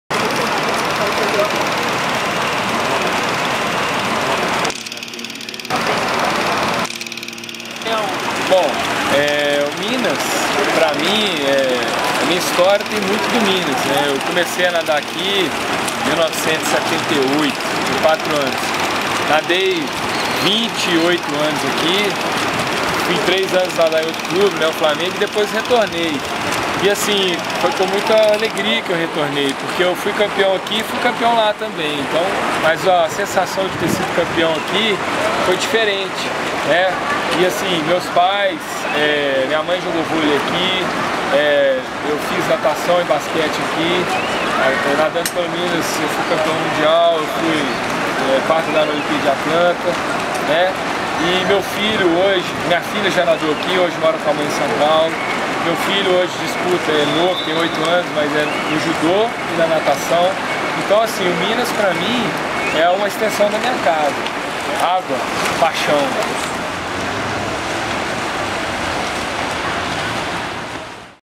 DIZ AÍ ANDRÉ
Conversa com André Carneiro, nadador e técnico do Minas Tênis Clube. Vídeo Realizado na oficina Processos Audiovisual Cocriativos em 2011 no Minas Tênis Clube.